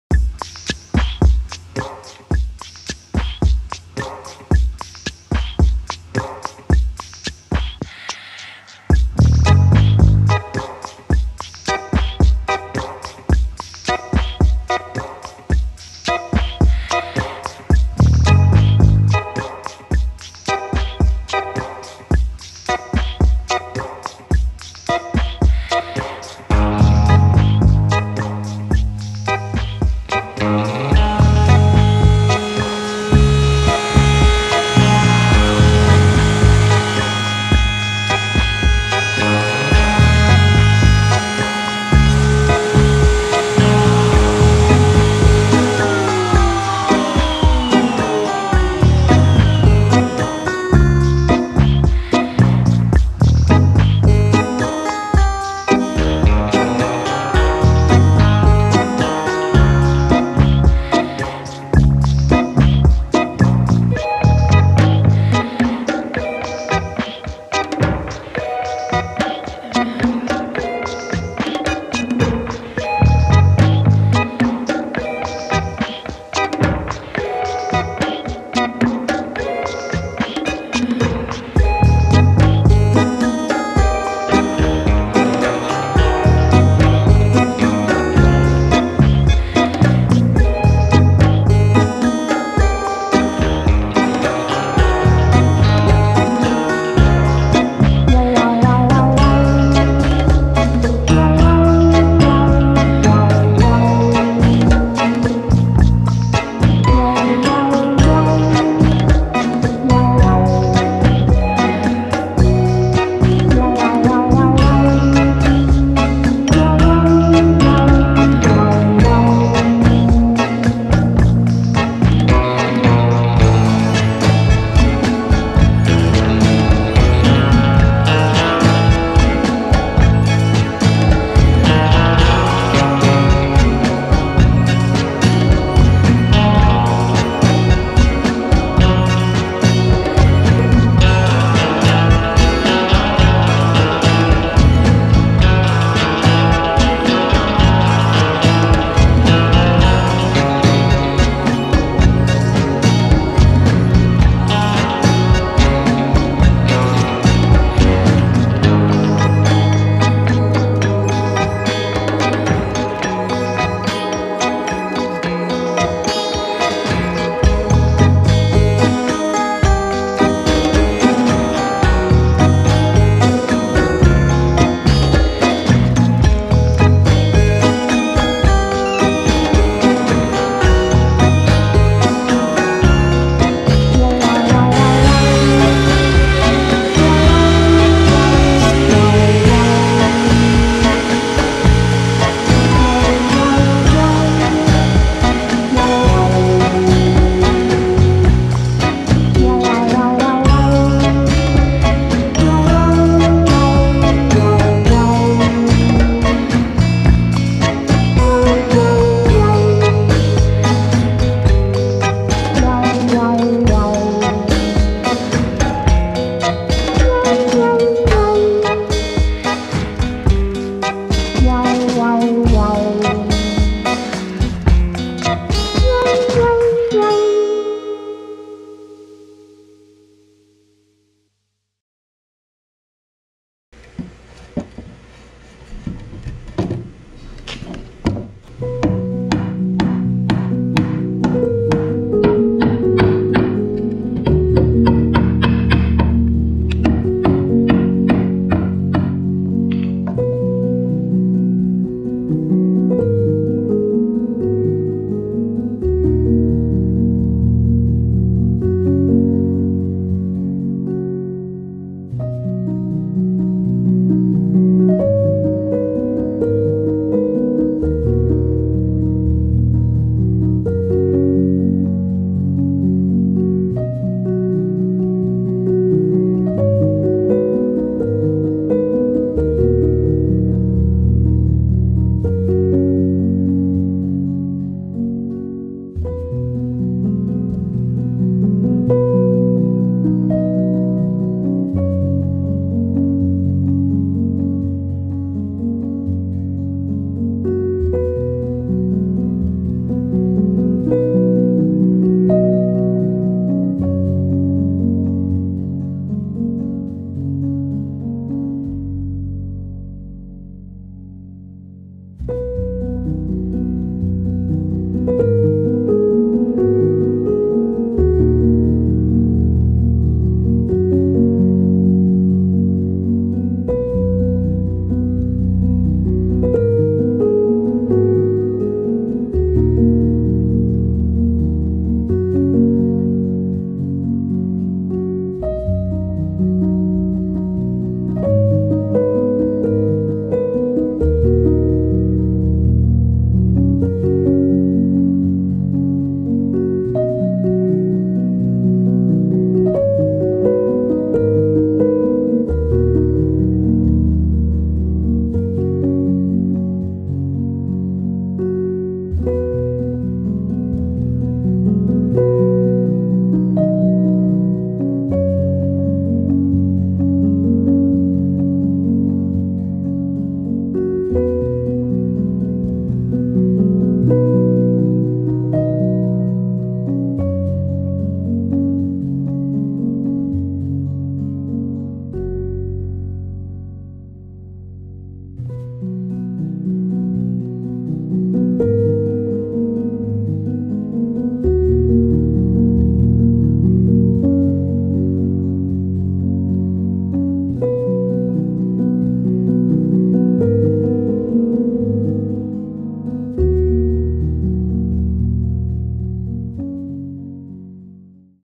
Cedar Hot Tub
This video is a guide to building a hot tub out of cedar.
This was a fun project and relatively simple.
Comment if you have any questions and I will try to answer them.